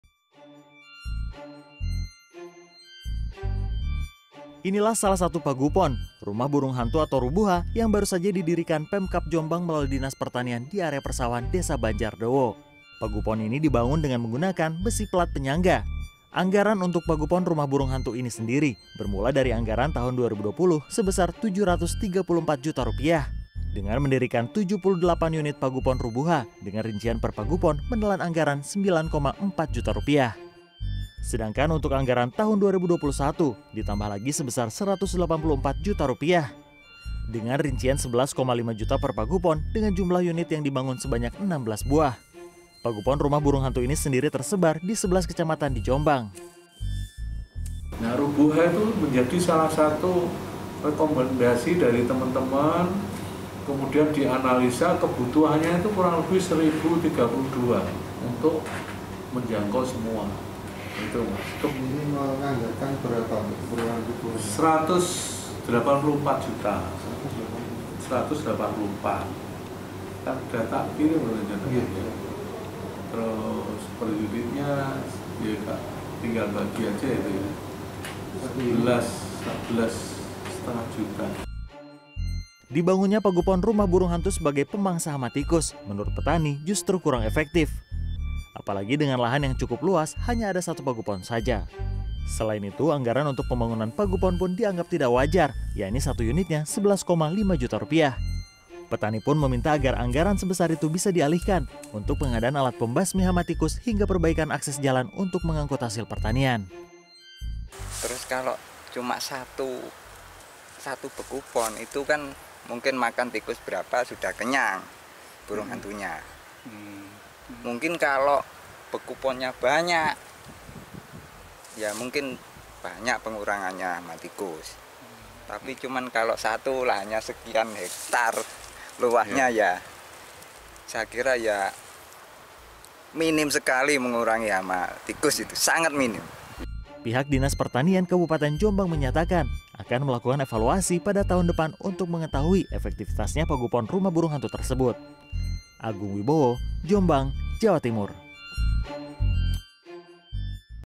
Pengadaan Rumah Burung Hantu Telan Ratusan Juta Rupiah
Pemerintah Kabupaten Jombang, Jawa Timur, melalui Dinas Pertanian menggelontorkan anggaran sebesar ratusan juta rupiah, untuk menanggulangi hama tikus dengan cara mendirikan pagupon rumah burung hantu atau rubuha.

Namun, para petani justru menyayangkan pendirian pagupon dan berharap anggaran sebesar itu bisa dialihkan untuk kebutuhan yang lain.